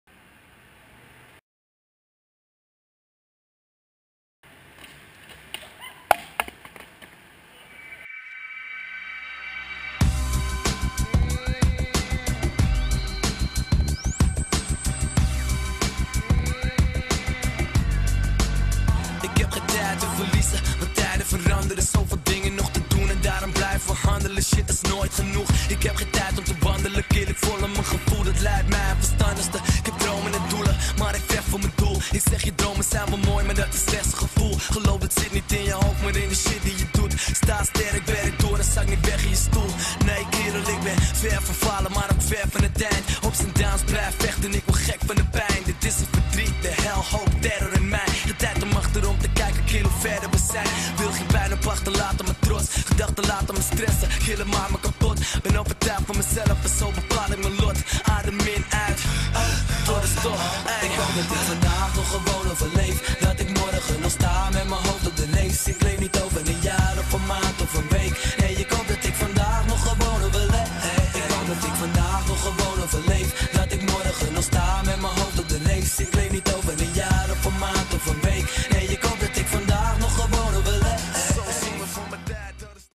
Bas Beugels - Rotterdam - 2013
One day of riding in Rotterdam.